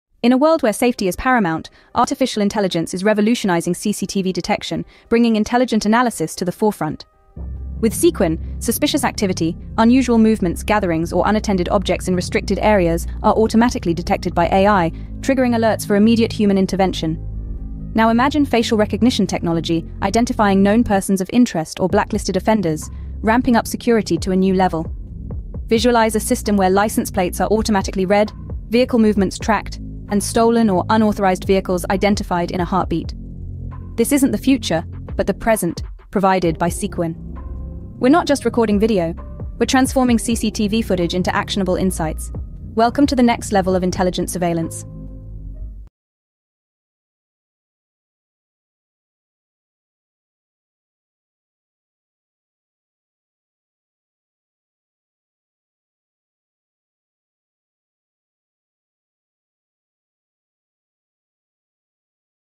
AI is revolutionizing CCTV detection | Sekuen
Discover how AI is transforming CCTV detection, adding a layer of intelligent analysis beyond passive video recording. From object detection and recognition to anomaly and behavior detection, AI-enhanced CCTV provides actionable insights like identifying suspicious activity, recognizing individuals of interest, and spotting loitering or vandalism. Predictive analytics forecasts potential threats, optimizes resource allocation, and identifies crime patterns. With AI-powered CCTV, organizations can reduce false alarms, improve incident response, and enhance data analysis for better security. Embrace AI and revolutionize your CCTV system's effectiveness, enabling proactive threat prediction and prevention.